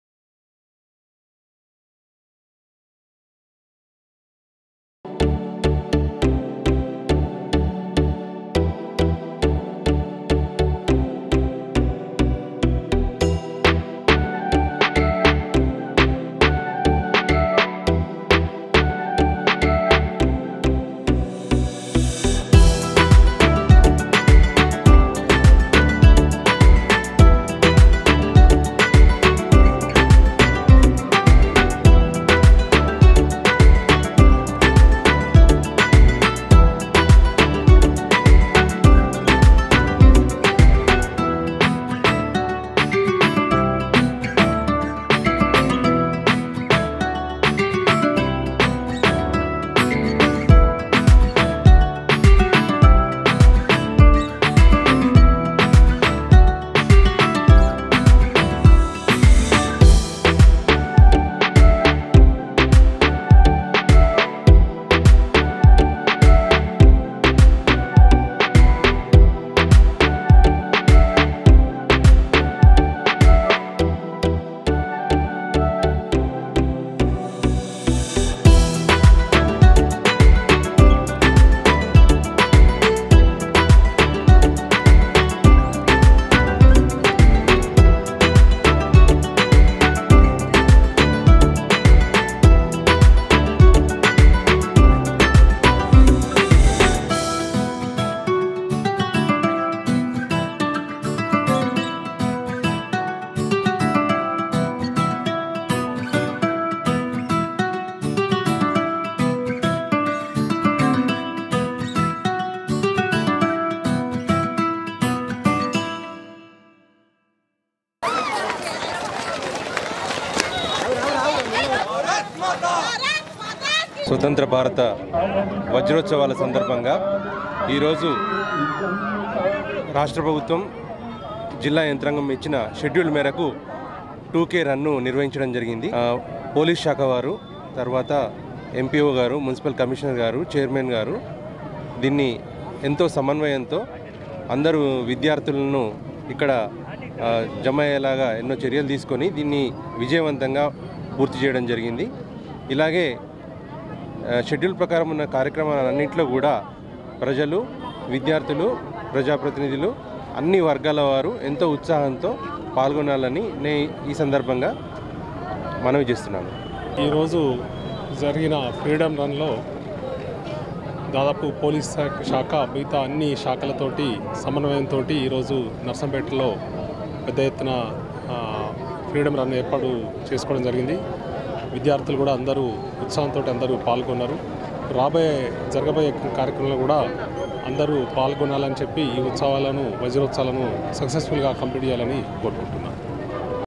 75వ స్వాతంత్ర వజ్రోత్సవాలలో భాగంగా2K రన్...
AZADI KAA AMRUTH MAHOSTAV as per the shedule program, today conducted Freedom Run at Narsampet town , in which RDO Narsampet, ACP Narsampet, Municipal chairperson, Municipal commissioner, MP DO, and students of all the schools participated...